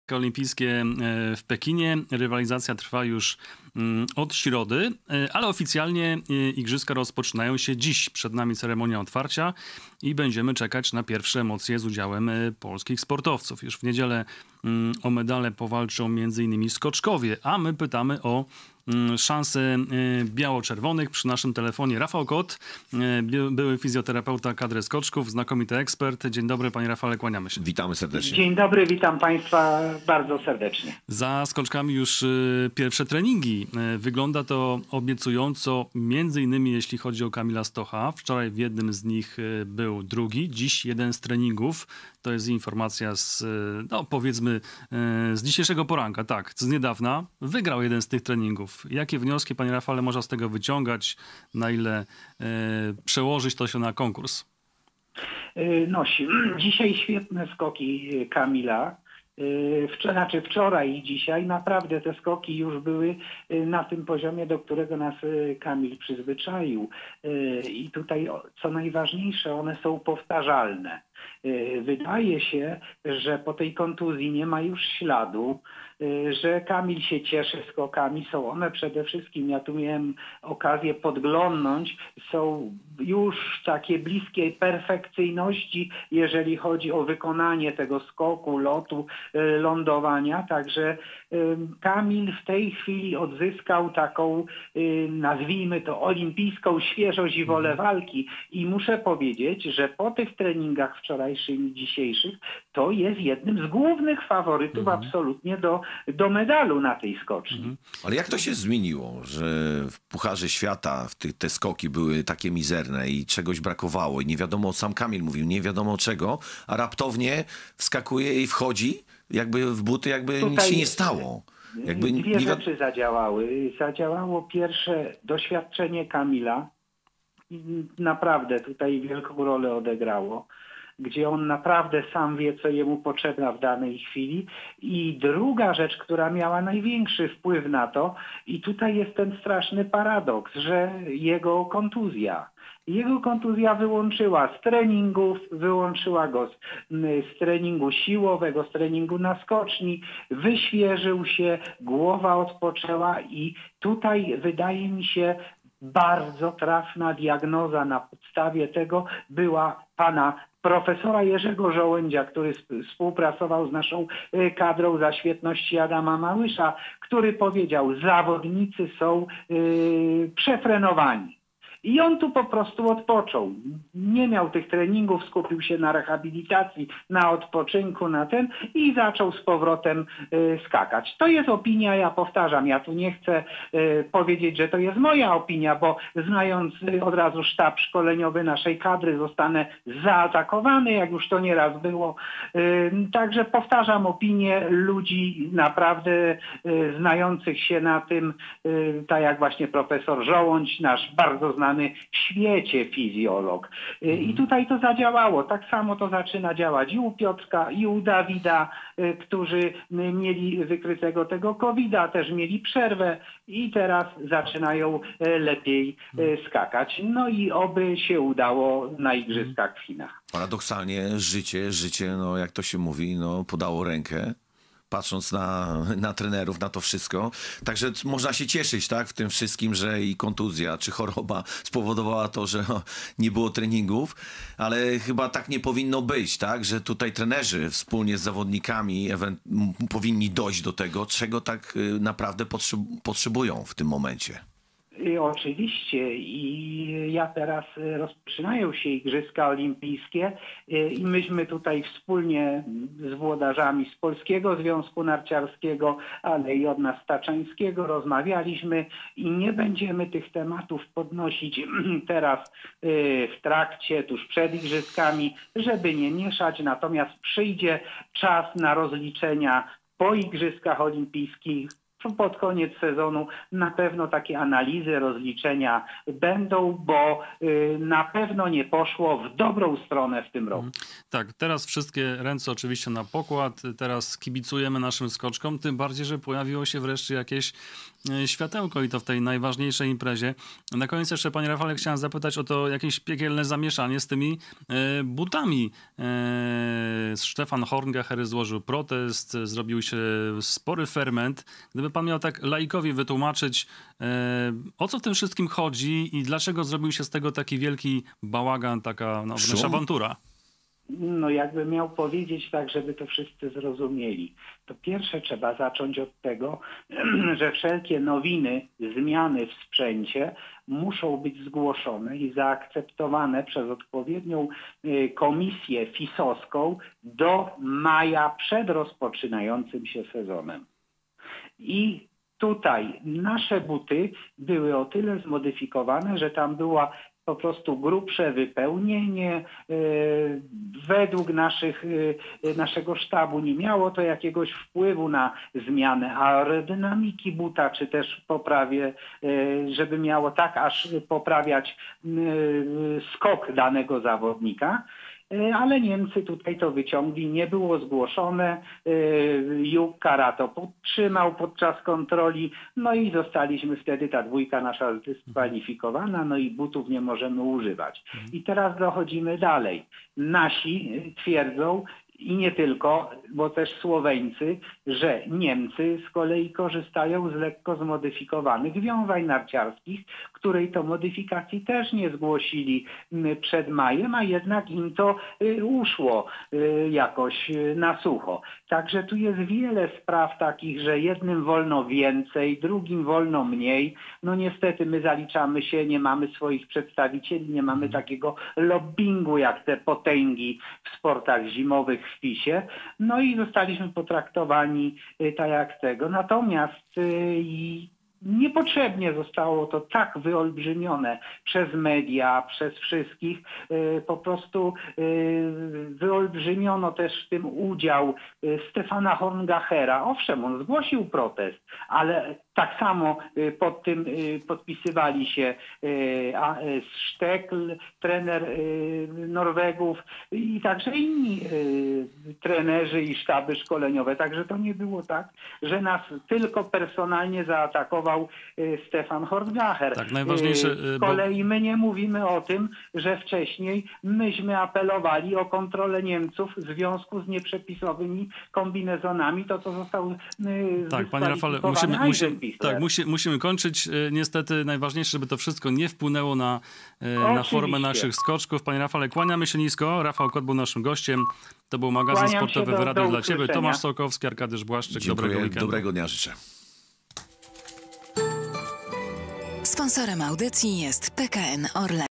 R. KOT o Kamilu Stochu, regeneracji, butach skoczków i nastrojach w kadrze
Gościem Arkadiusza Błaszczyka i Tomasza Sokołowskiego był Rafał Kot - były fizjoterapeuta kadry polskich skoczków.